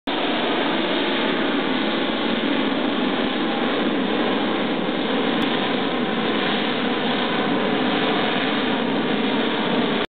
Harrier at Oshkosh
AV8B Harrier departing during a demonstration flight at AirVenture 2008 in Oshkosh, Wisconsin.